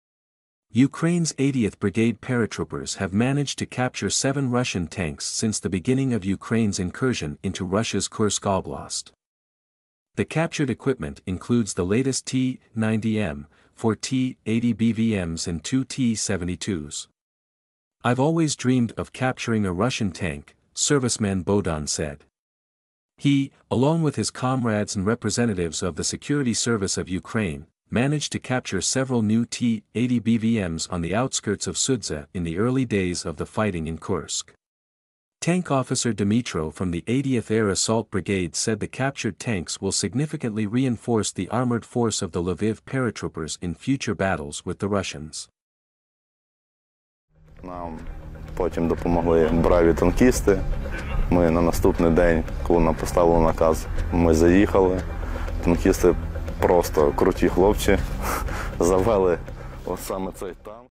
Ukraine's 80th Brigade capture and use 7 Russian tanks in Kursk Oblast
Ukraine’s 80th Brigade paratroopers have managed to capture seven Russian tanks since the beginning of Ukraine's incursion into Russia's Kursk Oblast.
Source: 80th Air Assault Brigade
Details: The captured equipment includes the latest T-90M, four T-80 BVMs and two T-72s.

"I've always dreamed of capturing a Russian tank," serviceman Bohdan said. He, along with his comrades and representatives of the Security Service of Ukraine, managed to capture several new T-80 BVMs on the outskirts of Sudzha in the early days of the fighting in Kursk.
Tank officer Dmytro from the 80th Air Assault Brigade said the captured tanks will significantly reinforce the armoured force of the Lviv paratroopers in future battles with the Russians.